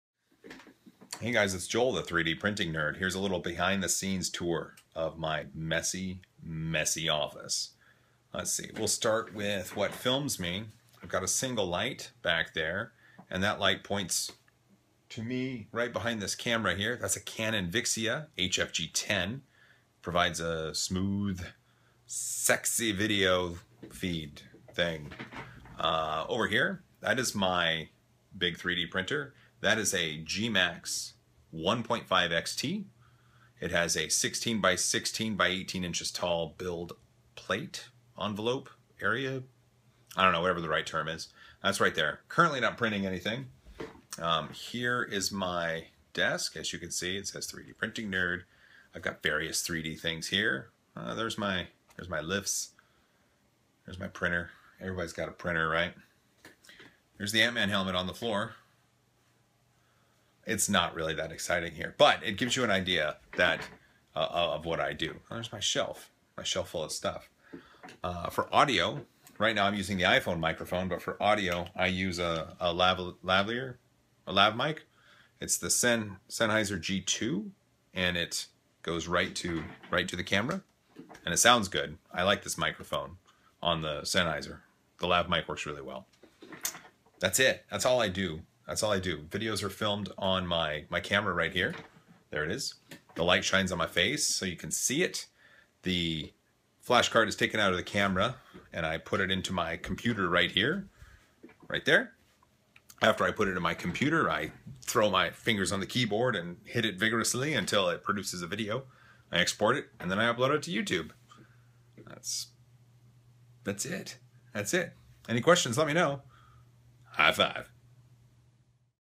Behind the Scenes of 3D Printing Nerd
I get asked how I film my videos and what I film them with. I go into detail in this video! I use a Canon VIXIA HFG10 and a Sennheiser G2 lav mic for the recording. I process the footage in Adobe Premiere and Adobe After Effects.